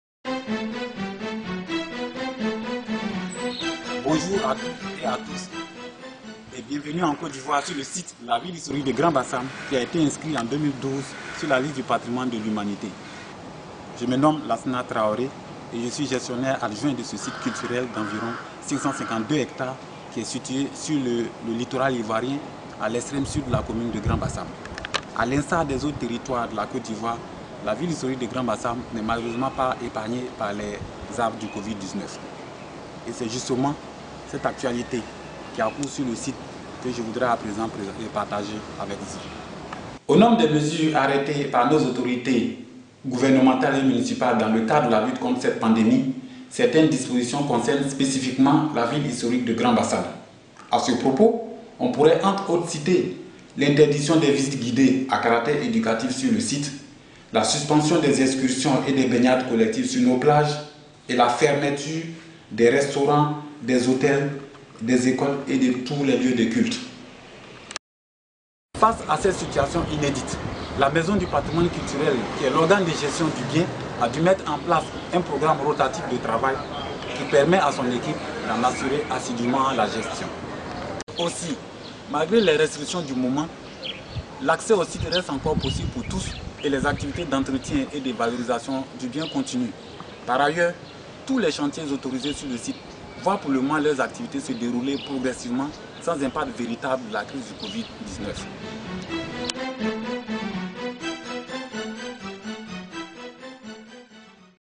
Lassiman Traoré : gestion de la ville historique de Grand Bassam pendant la crise du Covid-19
Témoignage de Lassiman Traoré, gestionnaire adjoint du site de Grand Bassam (Côte d’Ivoire) classé au patrimoine mondial de l’UNESCO, et ancien stagiaire de la formation internationale (2015) du Réseau des Grands Sites de France.